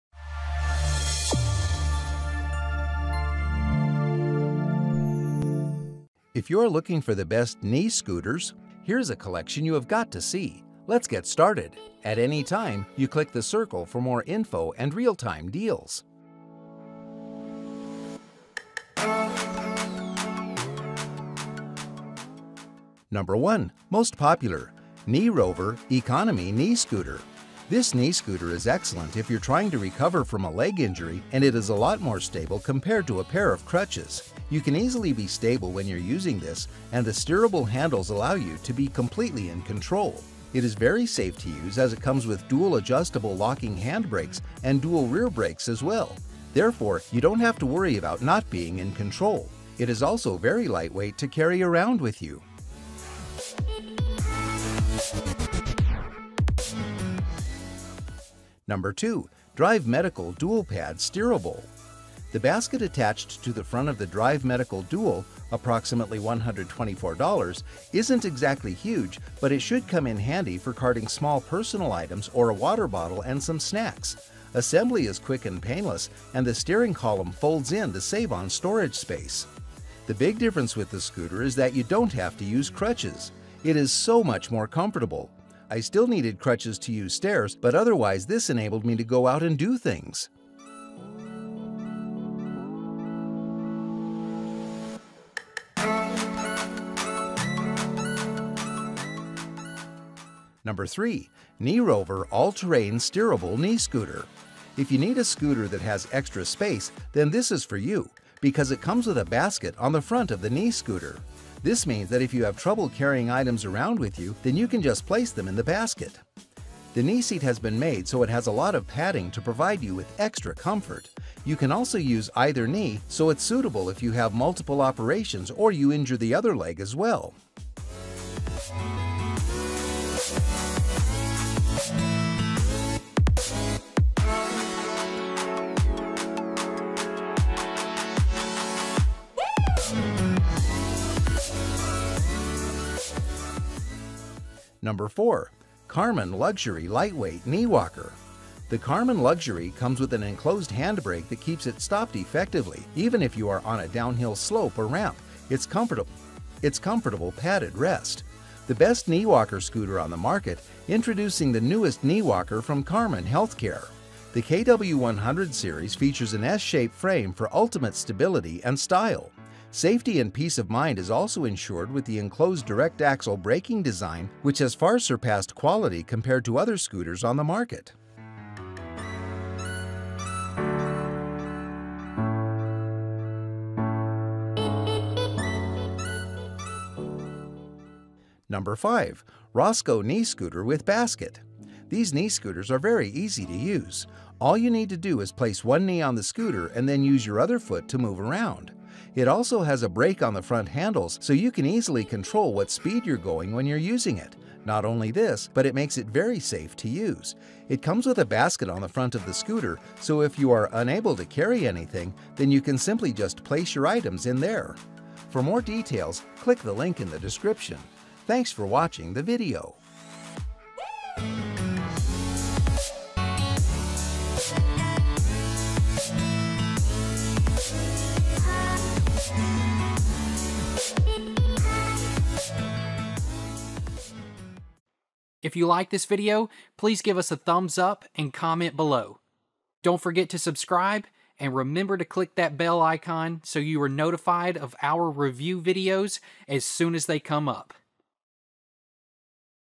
Top 5 Best Knee Scooters Review in 2022 | For Outdoor Use
Best knee scooters featured in this Video:
0:22 NO.1. KneeRover Economy Knee Scooter
1:00 NO.2. Drive Medical Dual Pad Steerable
1:40 NO.3. KneeRover All Terrain Steerable Knee Scooter
2:25 NO.4. Karman Luxury Lightweight Knee Walker
3:10 NO.5. Roscoe Knee Scooter with Basket

What is a knee scooter?
A knee bicycle or knee walker is a two, three or four-wheeled alternative as opposed to supports or a standard walker as an ambulation help. It is known by various names, for instance, knee napkin, knee cruiser, knee caddy, orthopedic bicycle, or leg walker.

During the time it has taken on various structures, from minimal wheeled contraptions fitting for indoor use to greater sturdier units prepared for use outside on grass or cleared surfaces. The current interpretation is commonly a lightweight, foldable arrangement that, with the knee flexed, supports the shin of the unusable extremity. The opposite foot arrives at the floor or ground, giving force.

What is the best knee scooter?
We strive to review as many products as we can, read real-time reviews and made our own ranking. We welcome all your comments.